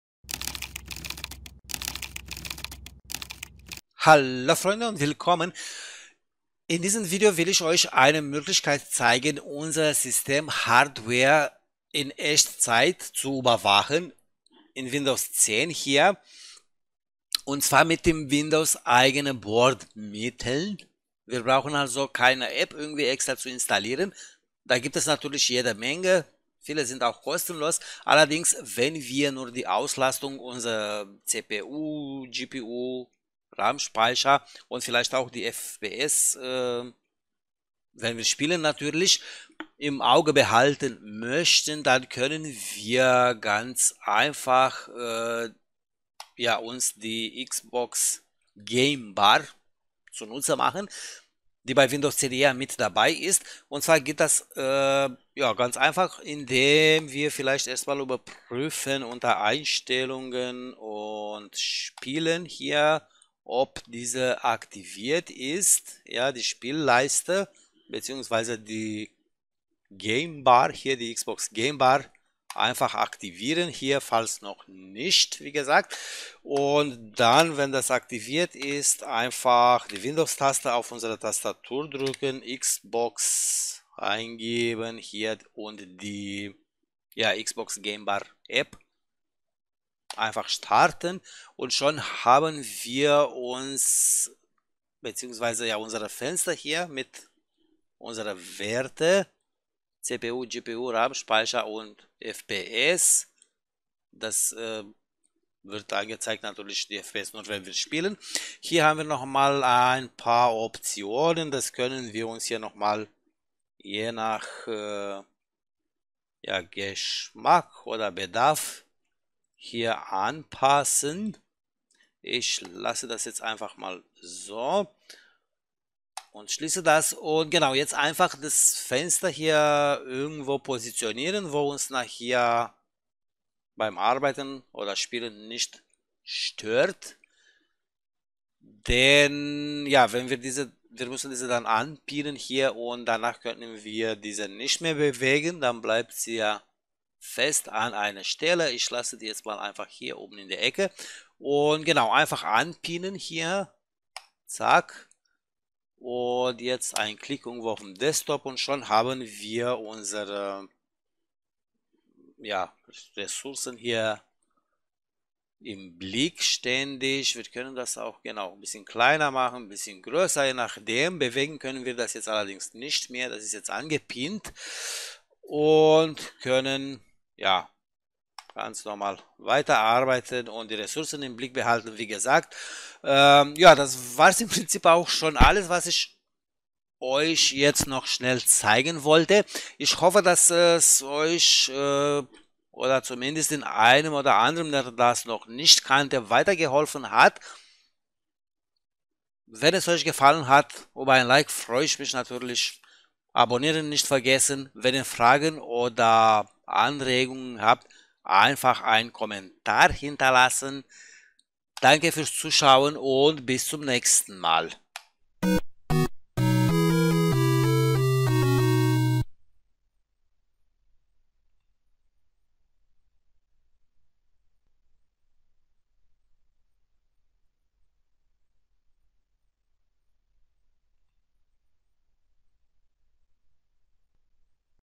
Windows 10: Systeminfos in Echtzeit im Auge behalten. Mit der Xbox Game Bar App.
Sony PlayStation 5

FIFA 21 ULTIMATE EDITION - [Playstation 4]
Xbox Game Bar nicht nur für Gamer nützlich.

* Amazon Partner-Link.